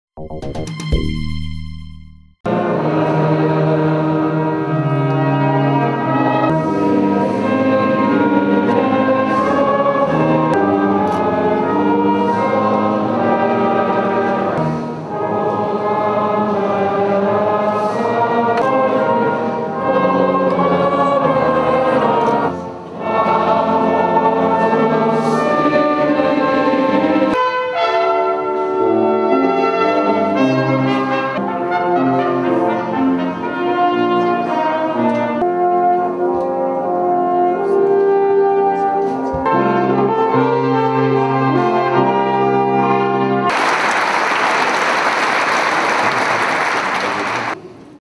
Noël en Irak pour les soldats américains
Les soldats américains ont fêté Noël en Irak. Un Noël plus calme que les précédents. Images. Durée 0:48